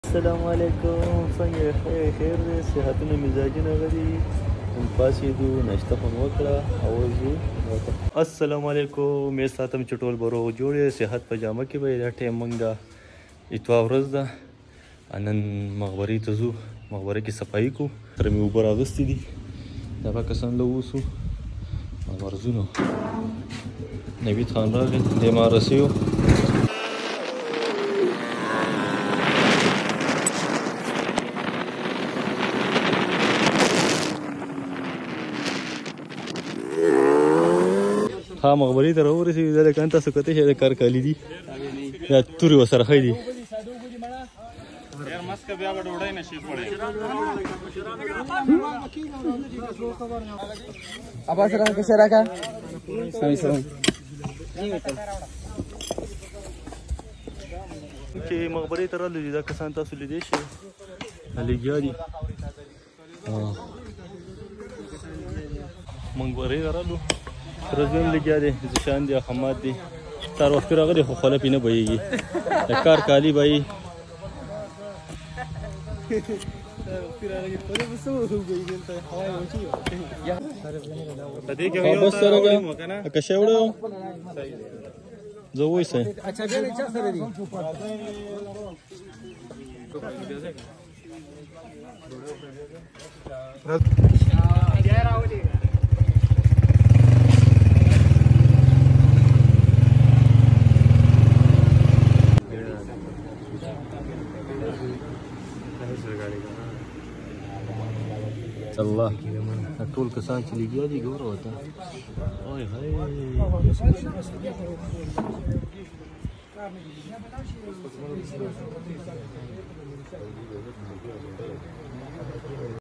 Working at Graveyard | Qabrestan ki kaam | Da maqbare kar | Pashto mini vlog
On Sunday our villagers together for cleanliness and working at our village graveyard.